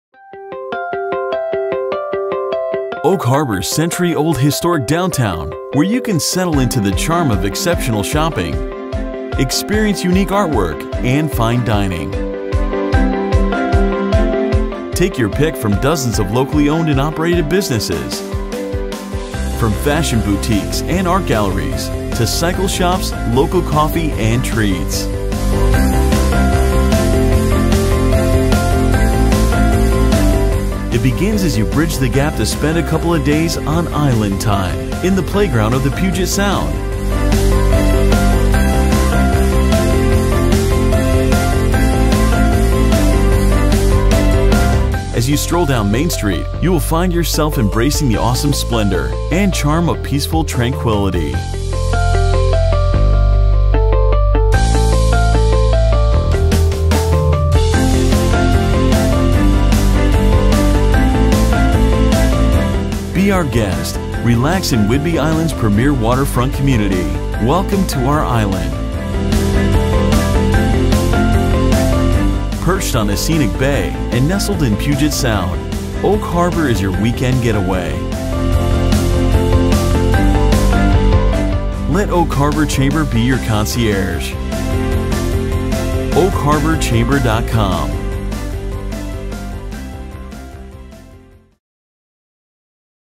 Welcome to Historic Downtown Oak Harbor!
A commercial filmed and edited for the Oak Harbor Chamber of Commerce-  Showcasing historic downtown & it's diversity is the focus.  From fine dining to art galleries, bike shops to candy stores, or just an evening stroll by the sea, Oak Harbor is a haven for laid-back tourists to escape and get on island time.